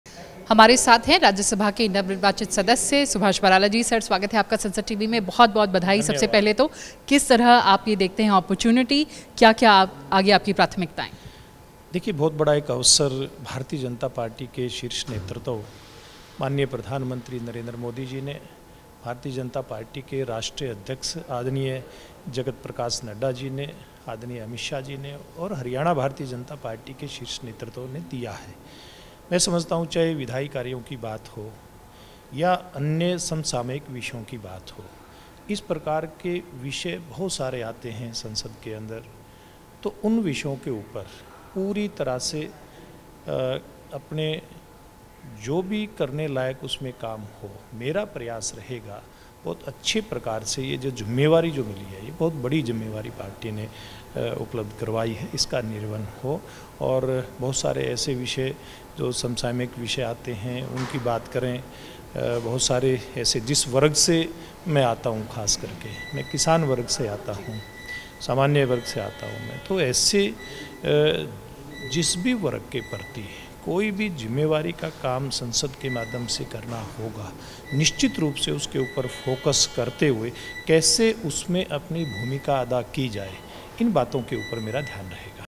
Subhash Barala, MP, Rajya Sabha speaks to Sansad TV | 04 April, 2024
Newly- Sworn in Members Speak to Sansad TV.